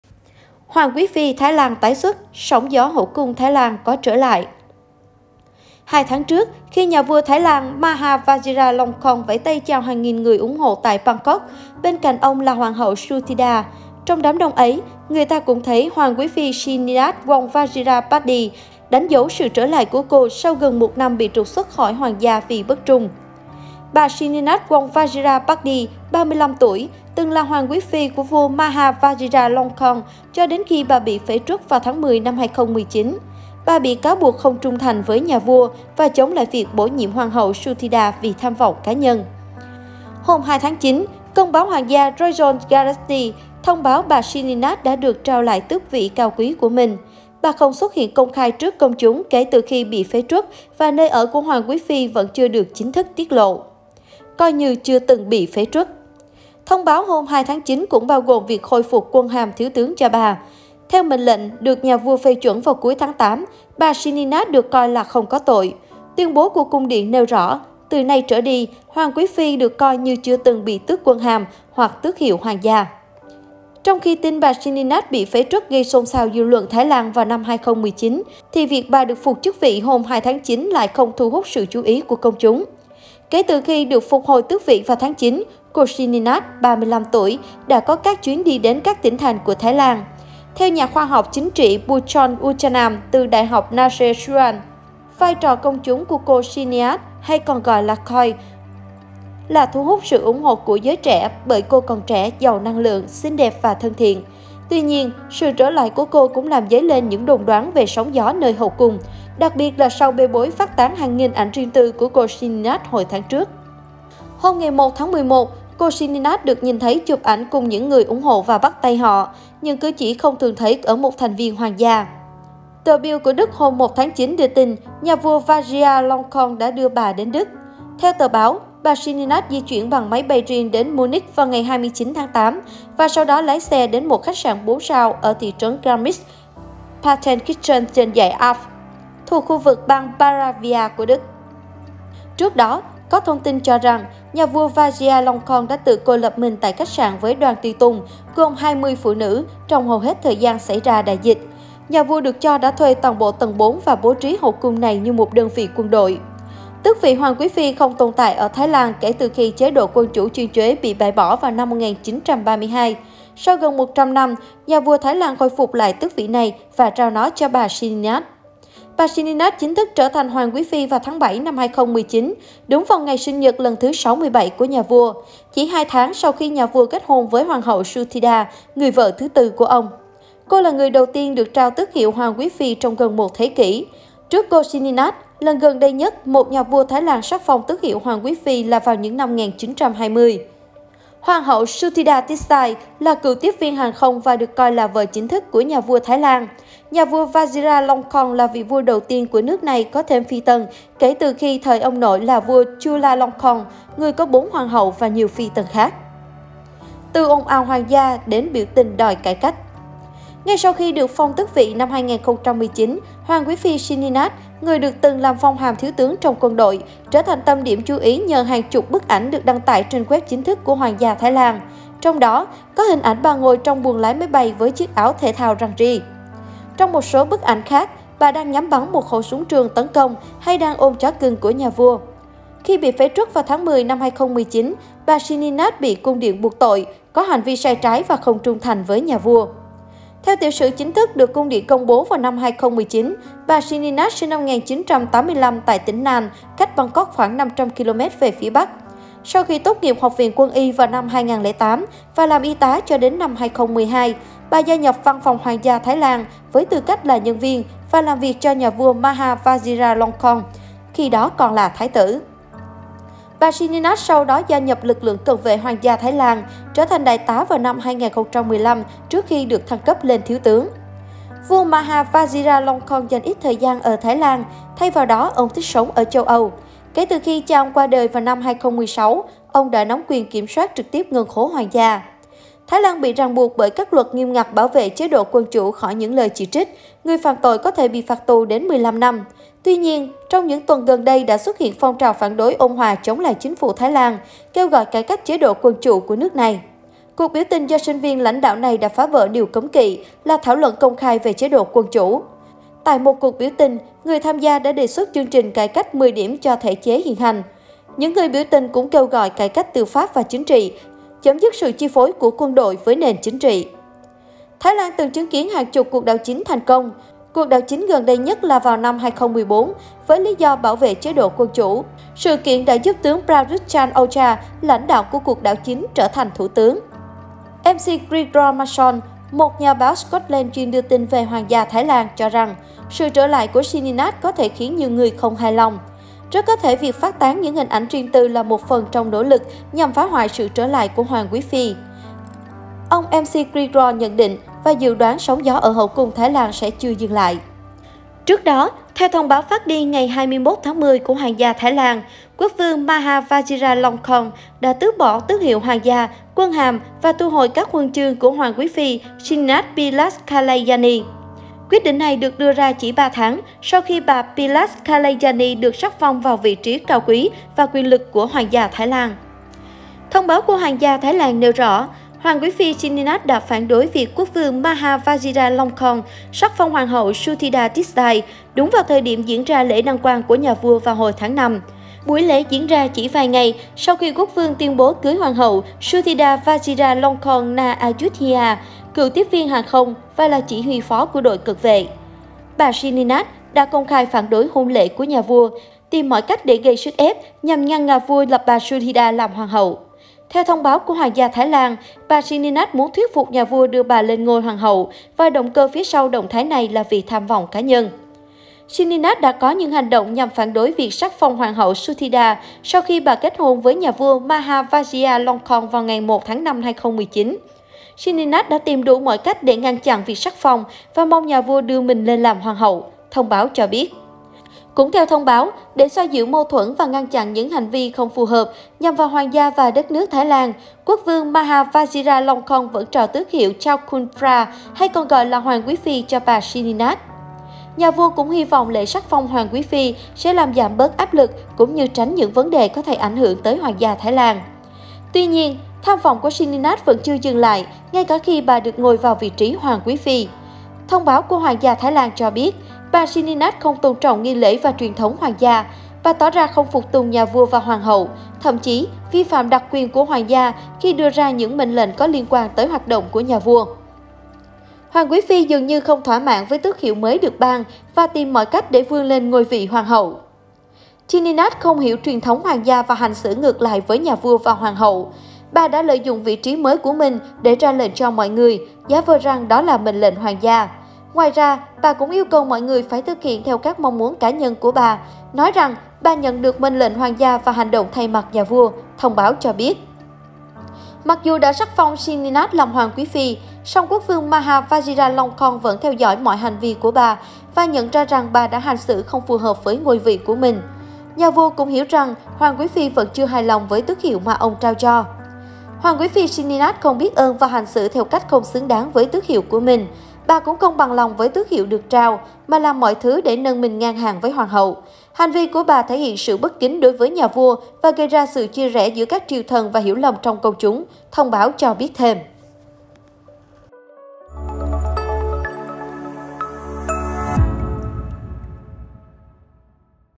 Sineenat Wong Vajira Pakdi - Hoàng quý phi Thái Lan tái xuất, sóng gió hậu cung Thái Lan có trở lại?
2 Tháng trước, khi Nhà vua Thái Lan Maha Vajiralongkorn vẫy tay chào hàng nghìn người ủng hộ tại Bangkok, bên cạnh ông là Hoàng hậu Suthida. Trong đám đông ấy, người ta cũng thấy Hoàng quý phi Sineenat Wong Vajira Pakdi, đánh dấu sự trở lại của cô sau gần một năm bị trục xuất khỏi hoàng gia vì bất trung.
Bà Sineenat Wong Vajira Pakdi, 35 tuổi, từng là hoàng quý phi của Vua Maha Vajiralongkorn cho đến khi bà bị phế truất vào tháng 10/2019. Bà bị cáo buộc “không trung thành với nhà vua” và “chống lại việc bổ nhiệm Hoàng hậu (Suthida)... vì tham vọng cá nhân".
Hôm 2/9, công báo hoàng gia Royal Gazette thông báo bà Sineenat đã được trao lại tước vị cao quý của mình. Bà không xuất hiện công khai trước công chúng kể từ khi bị phế truất và nơi ở của hoàng quý phi vẫn chưa được chính thức tiết lộ.